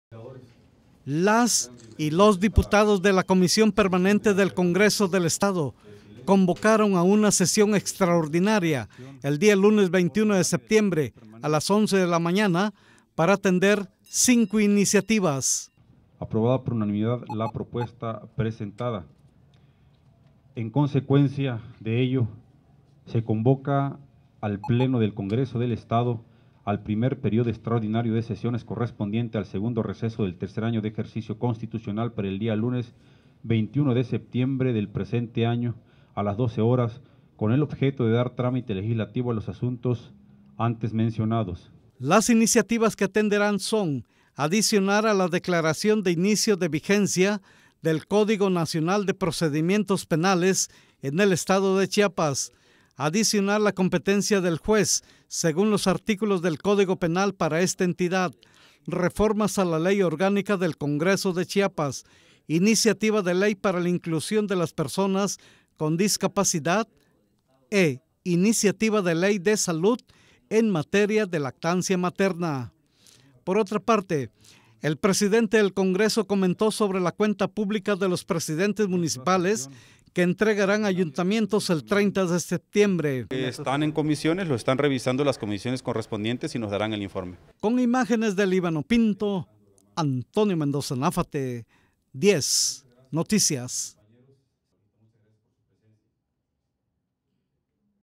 Comisión Permanente convoca a periodo extraordinario para atender iniciativas pendientes
Las y los diputados de la Comisión Permanente del Congreso del estado convocaron a una sesión extraordinaria el día lunes 21 de septiembre a las 11 de la mañana,  para atender cinco iniciativas.